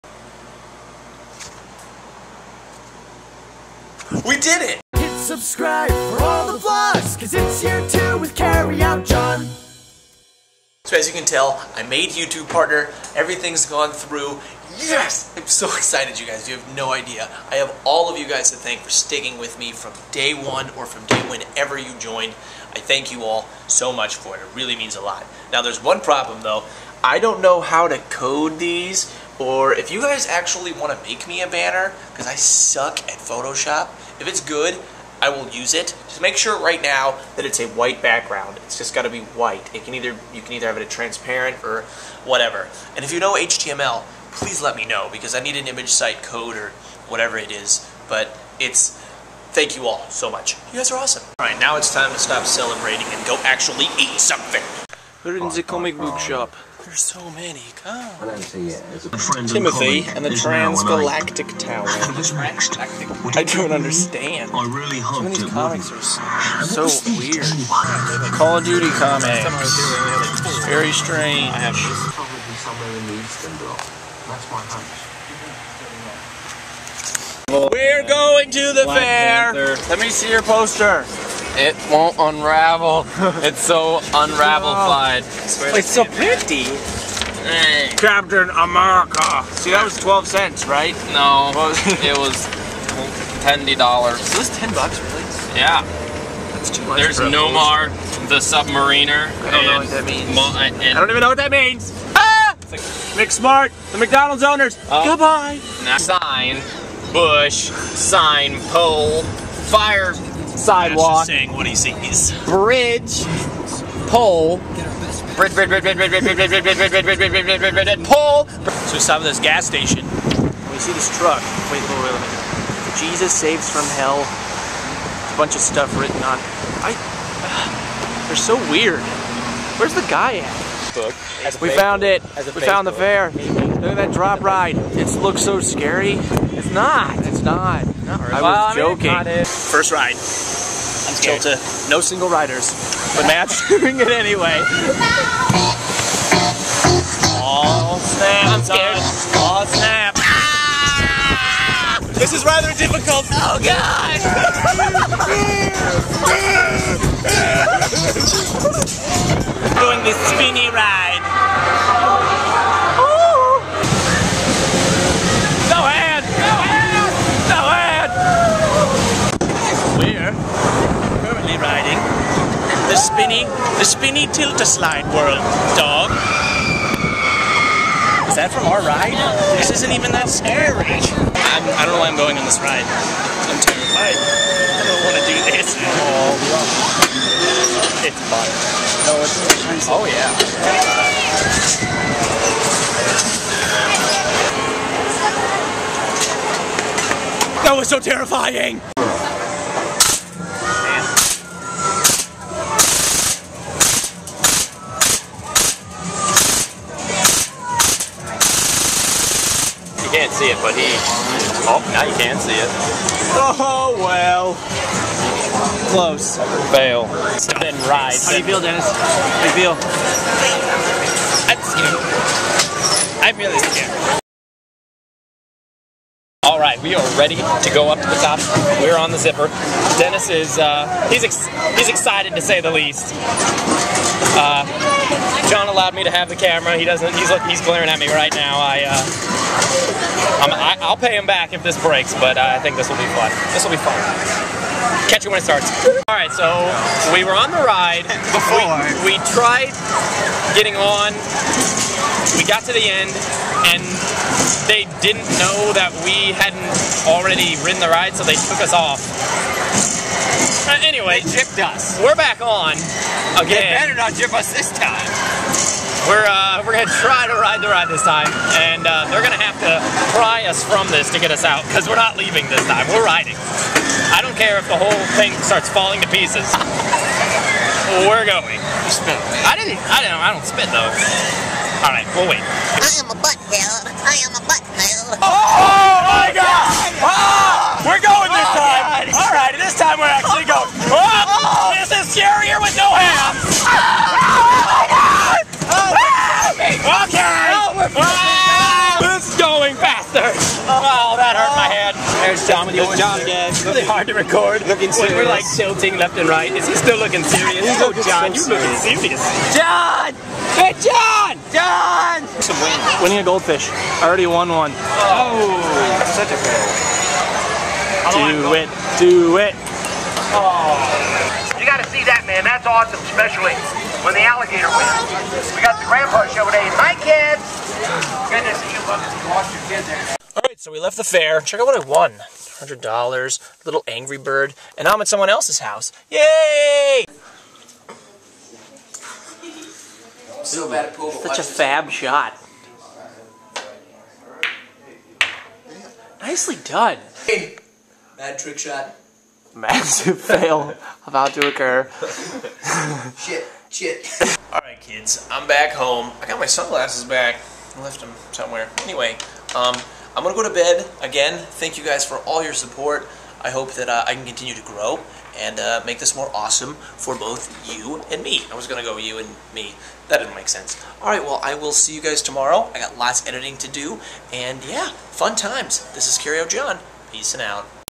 FAIR RIDE FREAKOUT! (8.4.11 - Day 10)
The local county fair was just recently set up, so we decided to hit that up today. Also, we went to the local comic book shop, followed by more awesome chilling.